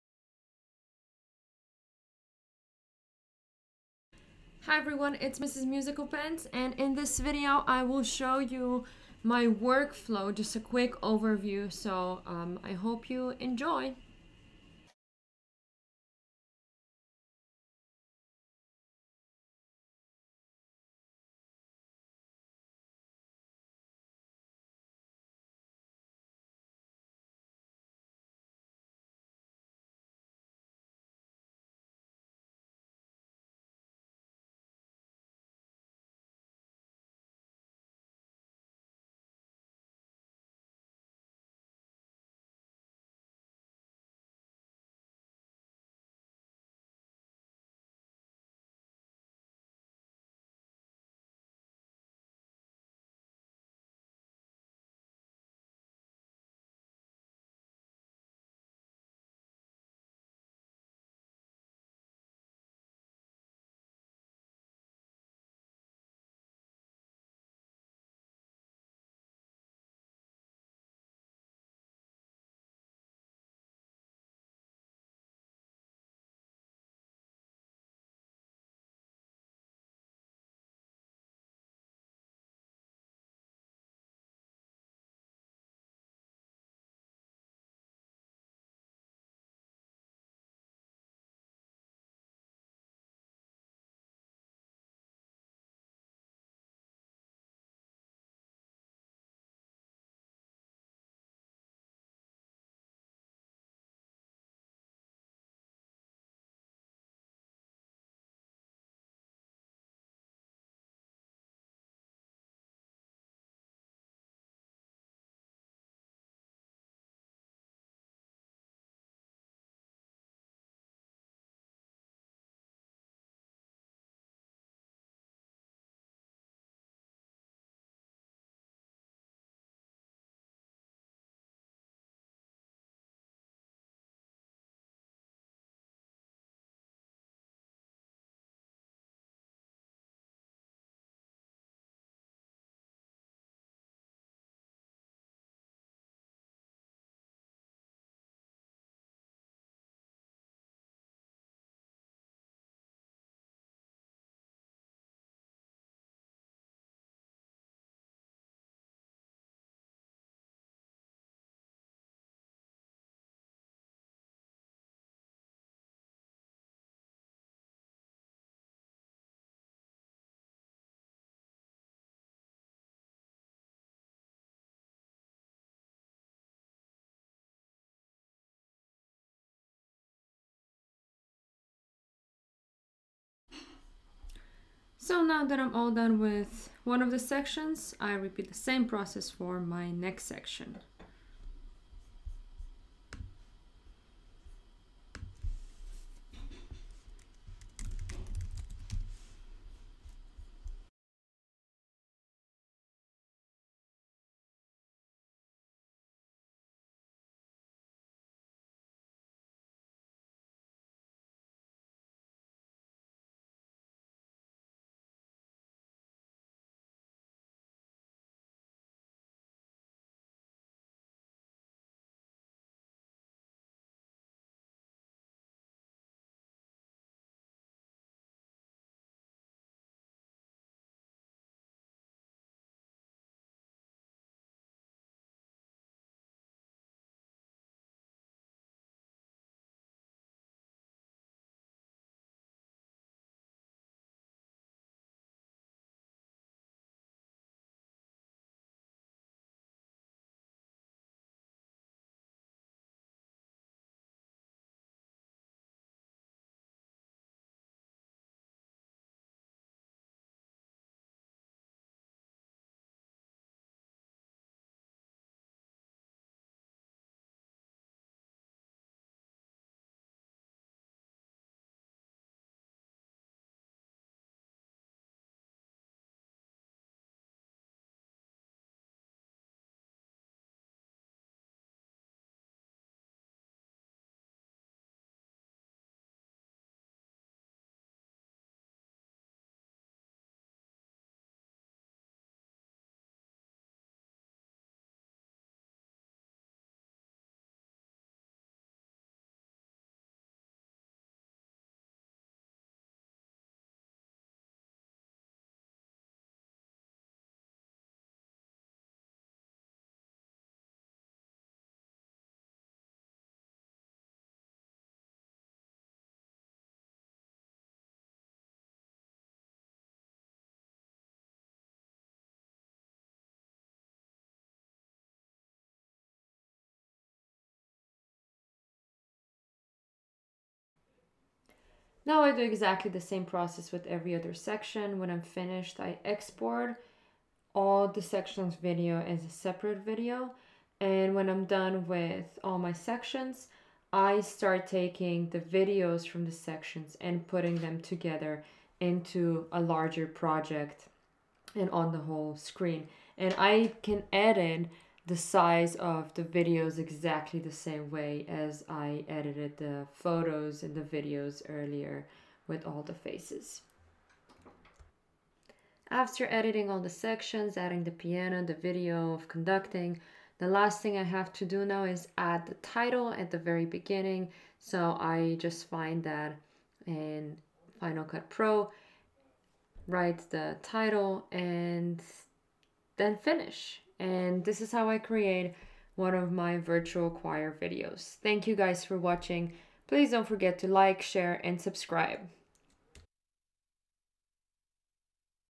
Virtual Choir in FCP - Quick overview
Check my other FCP videos for details. This is a fast workflow overview of how I create a Virtual Choir in Final Cut Pro on MacOS. Follow along as I create a virtual choir video.